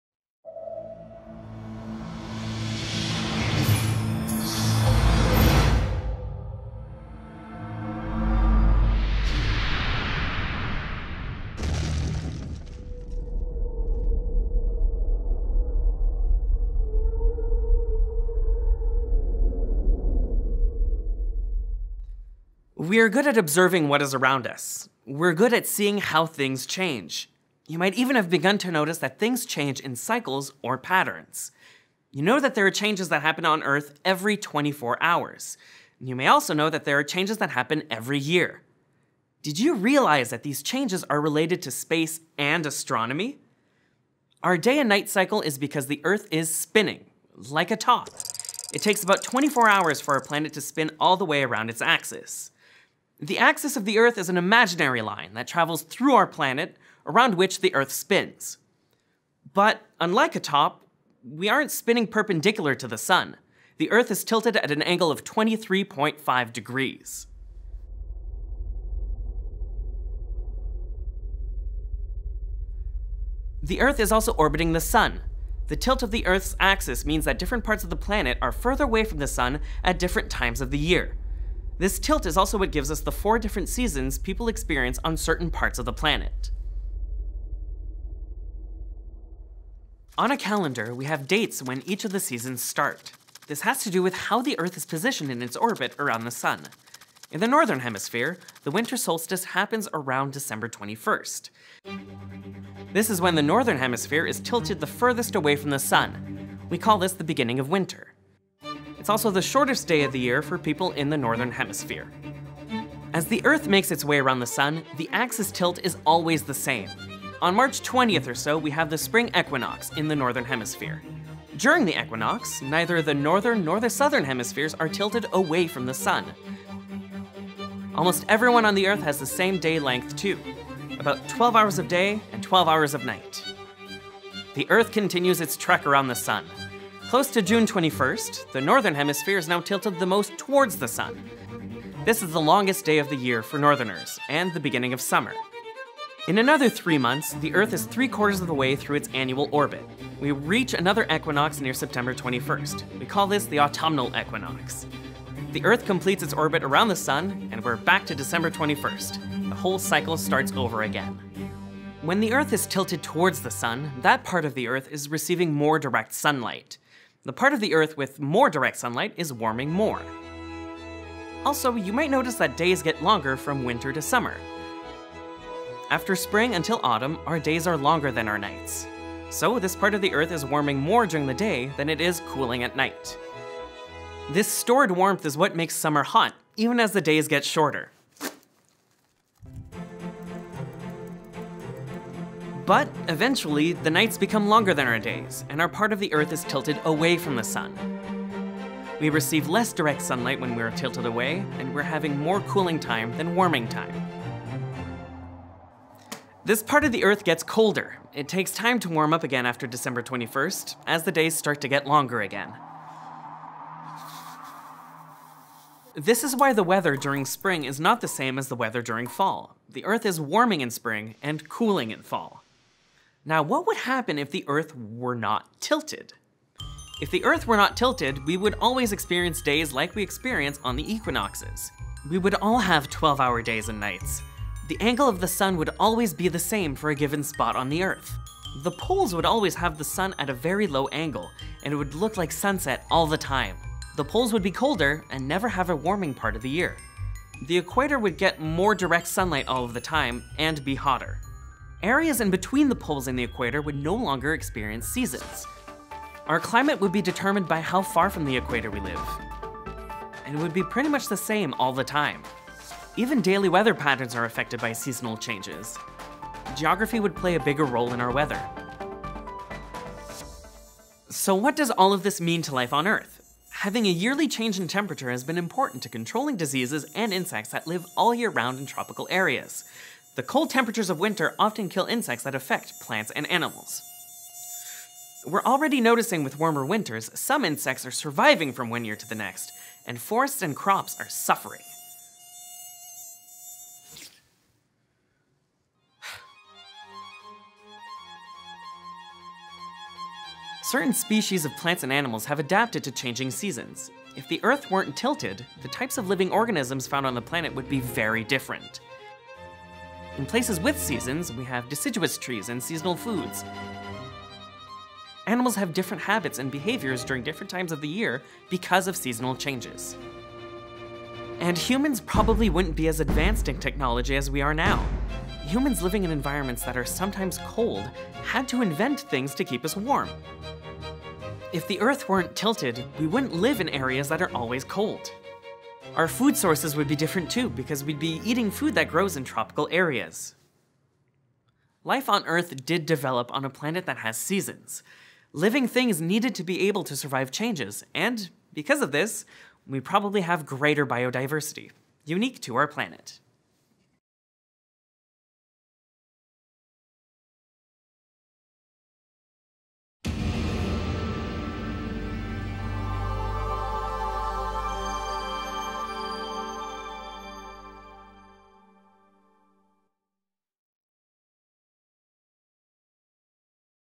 Science for the Classroom: Seasons
Did you know that the change in seasons are related to space and astronomy? Learn about how the Earth’s tilt affects plants, animals and our daily weather patterns as our planet travels around the sun each year.